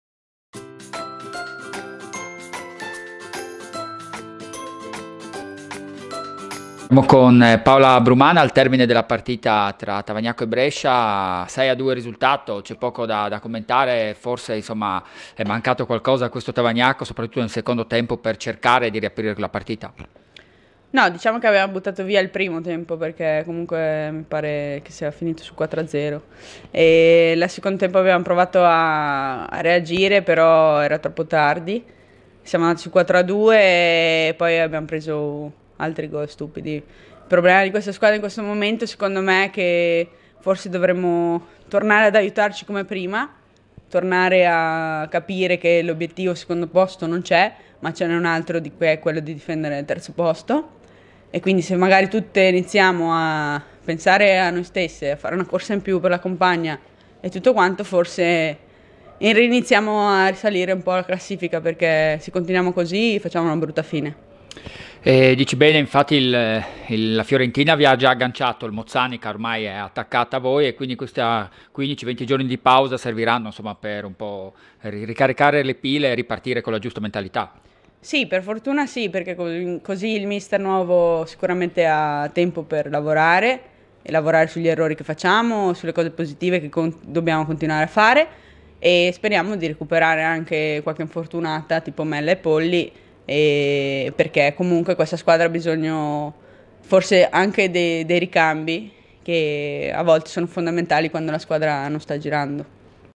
Paola Brumana post Tavagnacco-Brescia 2-6
Interviste dopo gara di Tavagnacco-Brescia. Serie A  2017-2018.